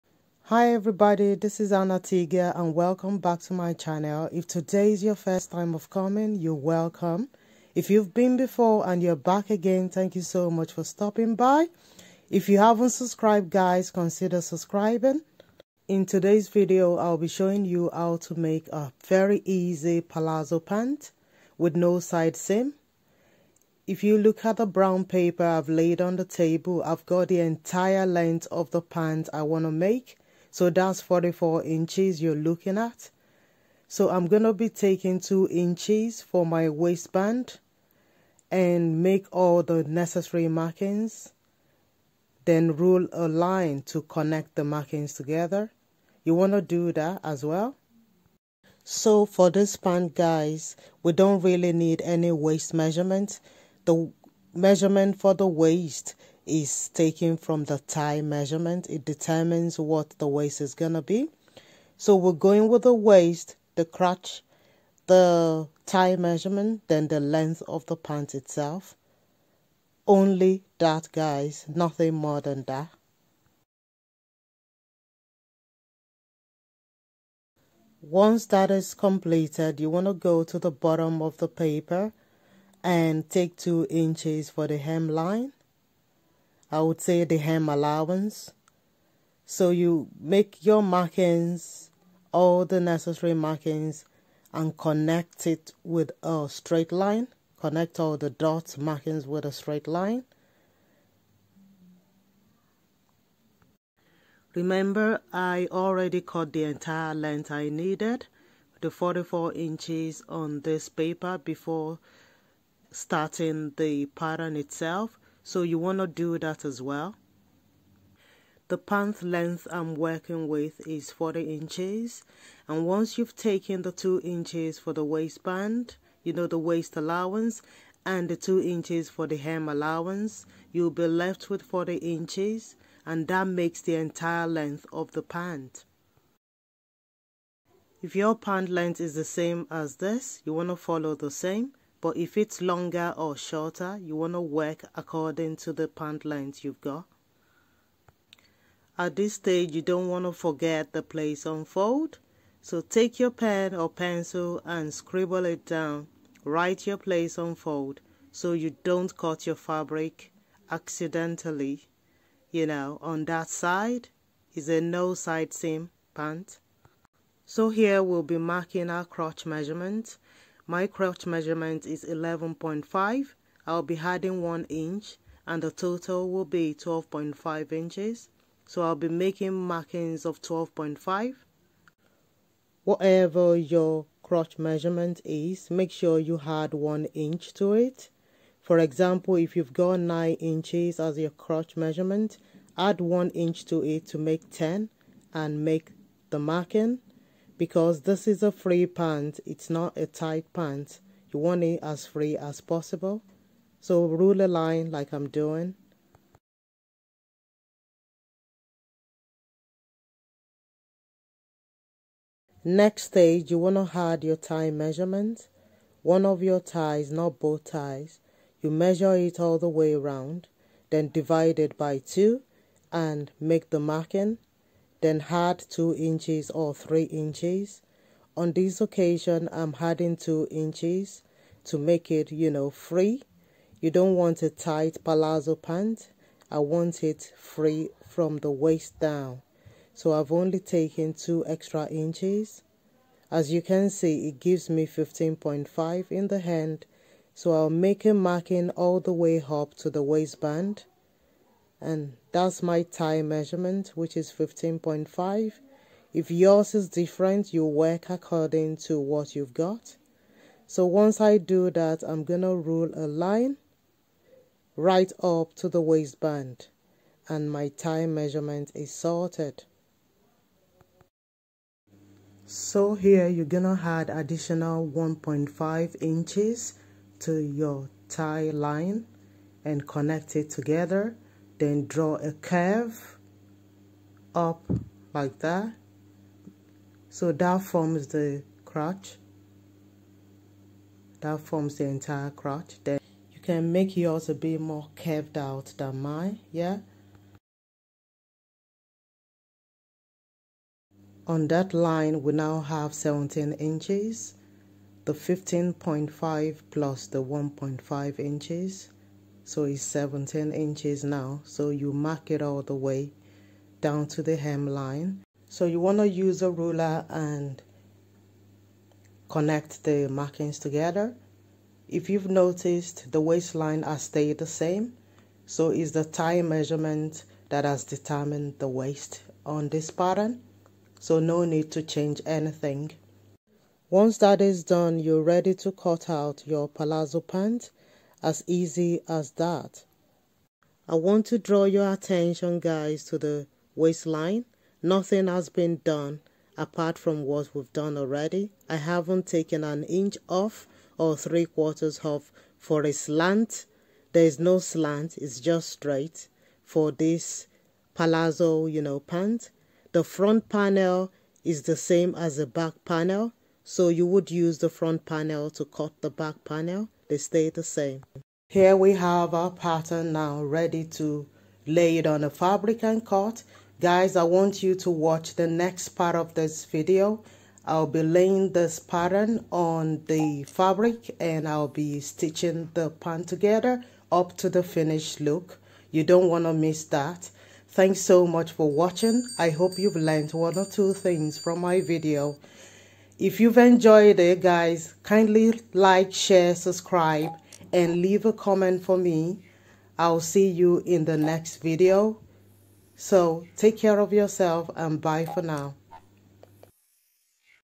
Palazzo Pant Pattern Drafting [No Side Seam] || Wide Leg Trouser
Easy Palazzo Pant project for beginners. DIY in 30 minutes from the comfort of your home.